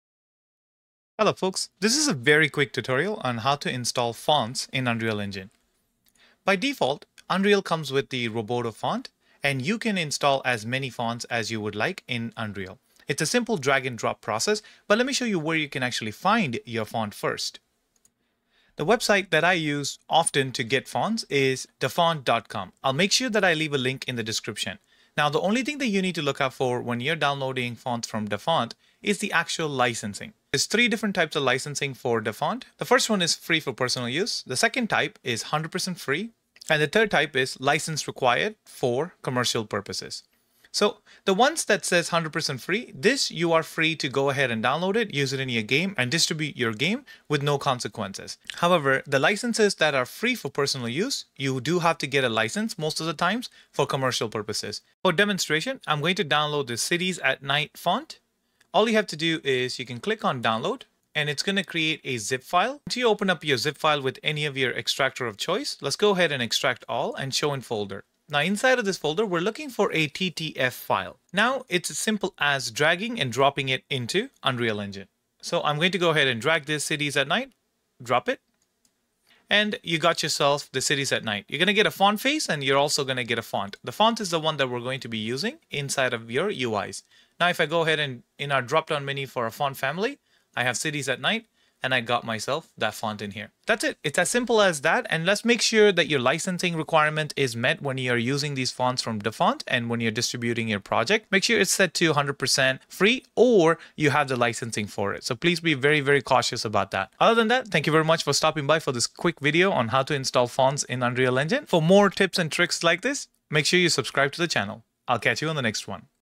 How to Install New Fonts in Unreal Engine - Quick Tutorial
Learn how to quickly and easily install new fonts in Unreal Engine! This step-by-step guide will show you how to add custom fonts to your projects, ensuring your UI stands out with unique typography. Perfect for beginners and experienced developers alike!